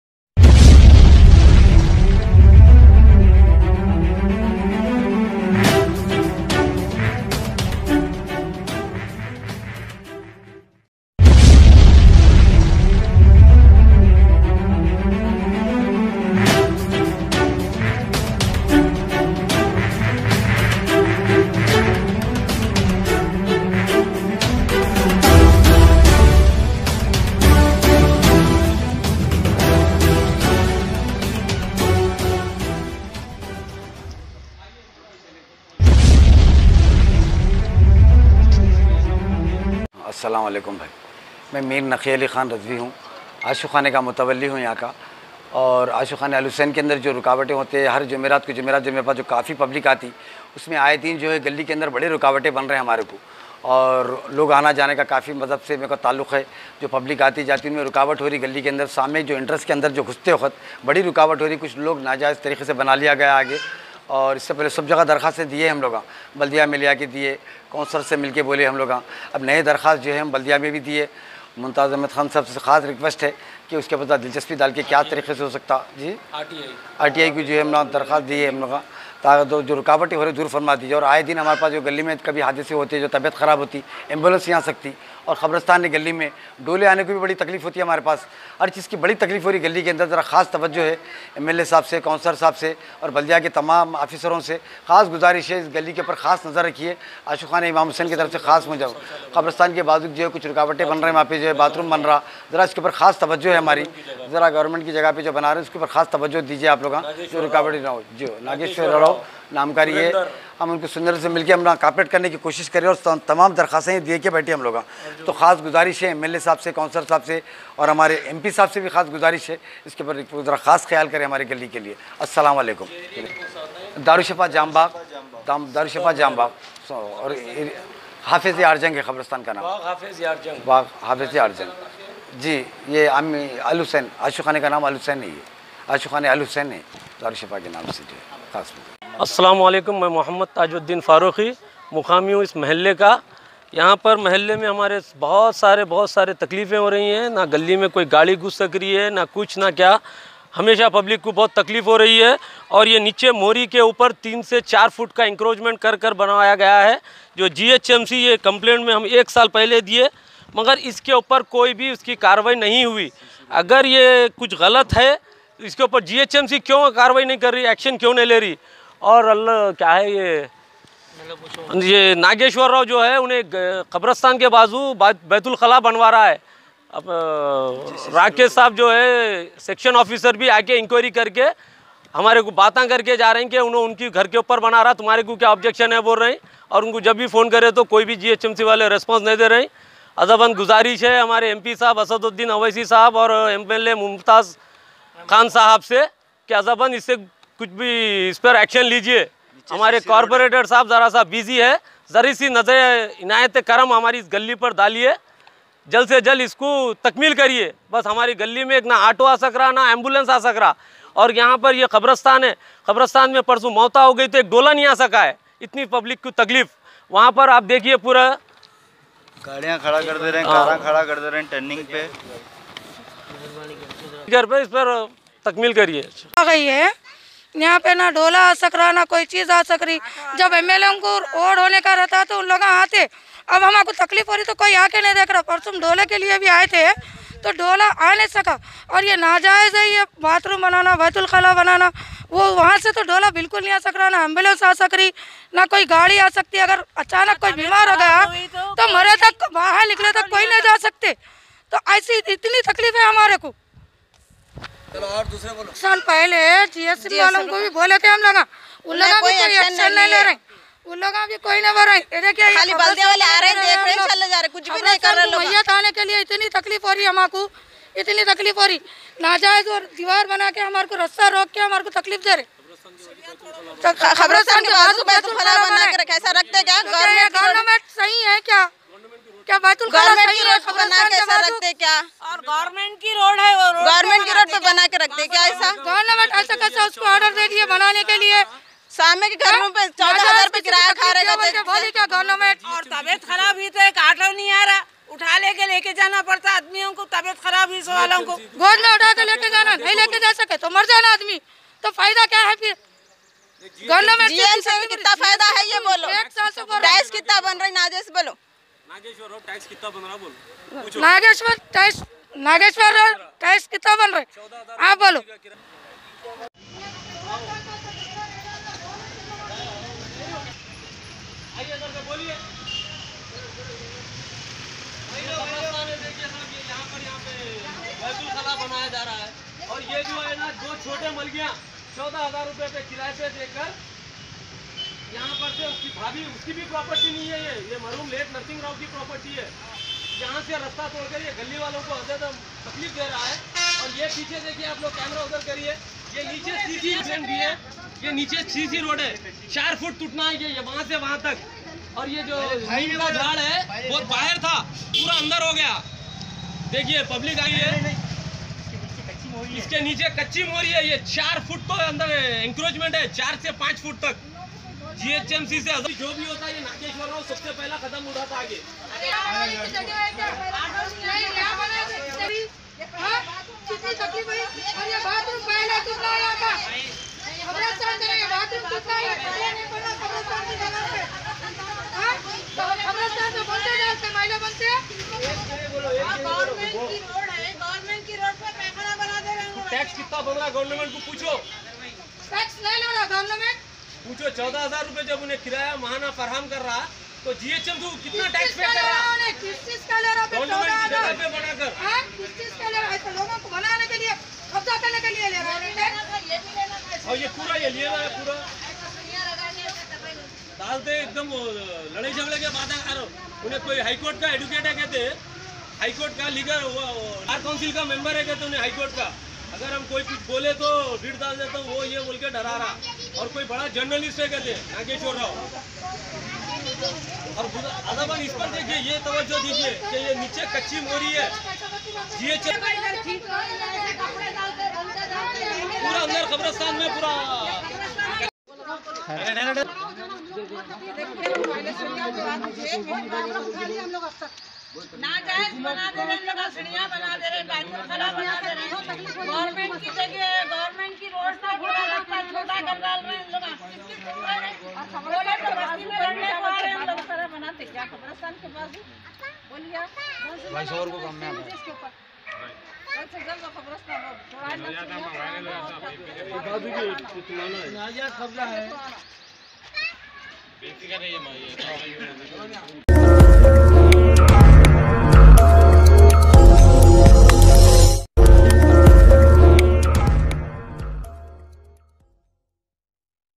Illegal encroachment in Public and Graveyard property at Darulshifa | RIGHT MEDIA NEWS
Illegal encroachment in Public and Graveyard property at Darulshifa

Two brothers named Nagesh & Surrender created illegal Staircase in Public Property, which also obstructs the path to graveyard. They also create law and order issues in the area.

GHMC Please take immediate action and remove the illegal encroachment..!! ||2-3-2019|| HYDERABAD || REPORT BY : MOHAMMED SHAHBAZ. ||CHIEF JOURNALIST OF RIGHT_MEDIA_NEWS||.*